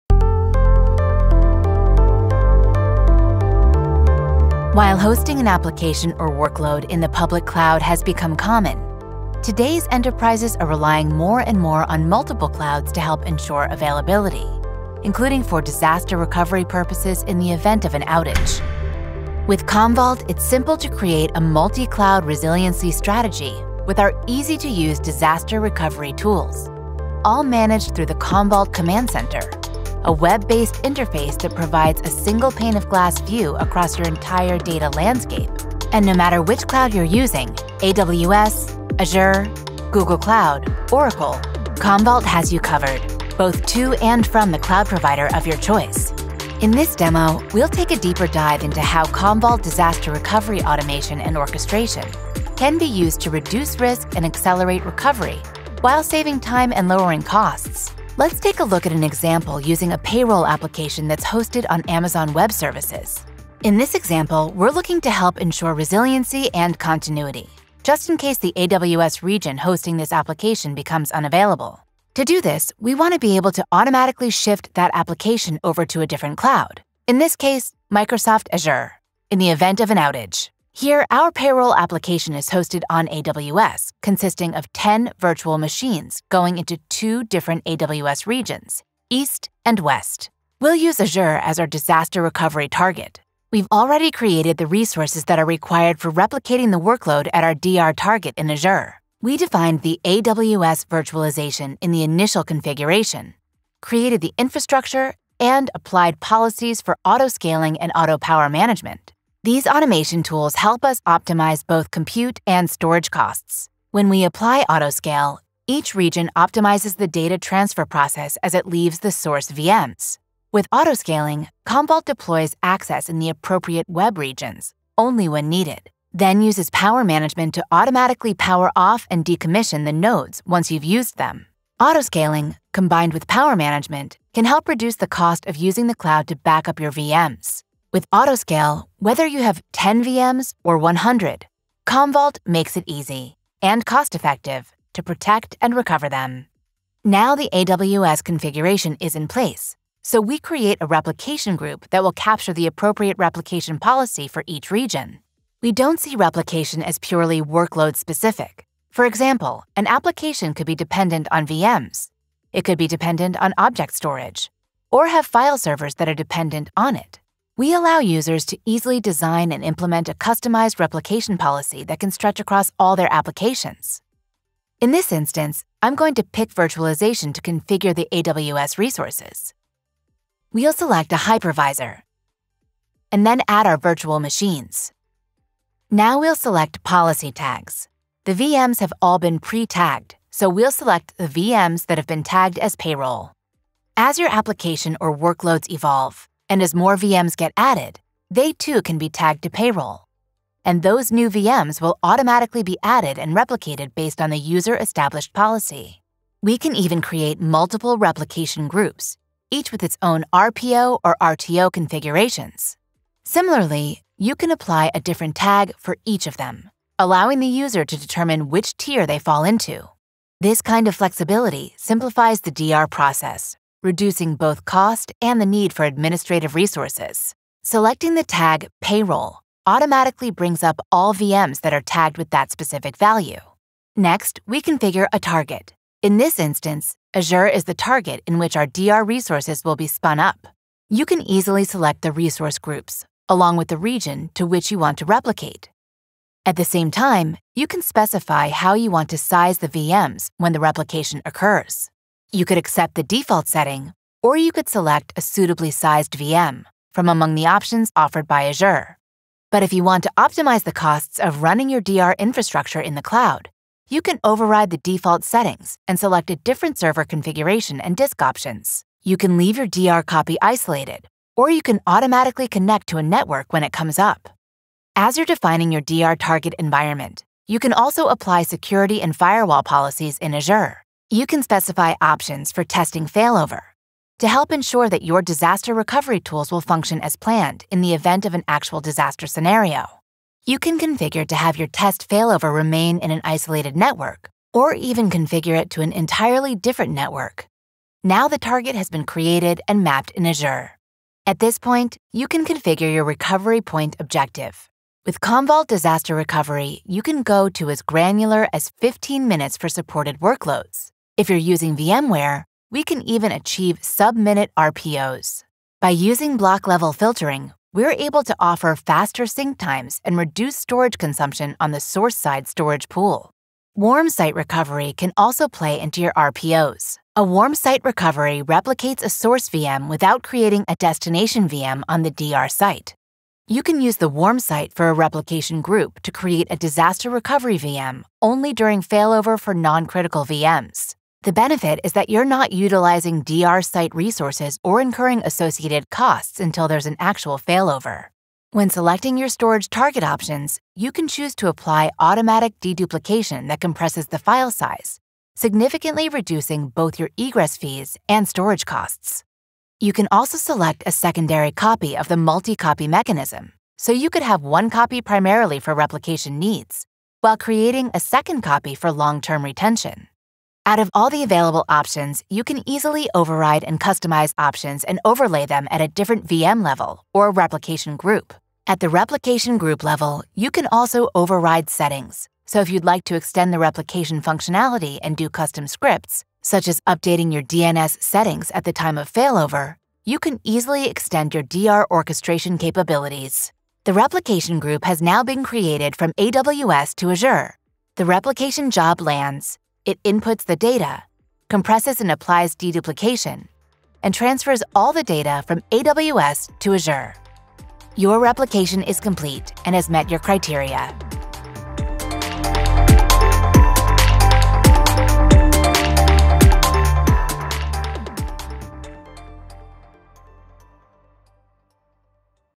Demo - Reduce Risk & Recovery time with Commvault (Automation) Part 1
While hosting an application or workload in the public cloud has become common, today’s enterprises are relying more and more on multiple clouds to help ensure availability, including for disaster recovery purposes in the event of an outage. In this demo, we’ll take a deeper dive into how Commvault Disaster Recovery automation and orchestration can be used to reduce risk and accelerate recovery while saving time and lowering costs. Part 1 covers how to create a multi-cloud resiliency strategy with our easy to use Disaster Recovery tools.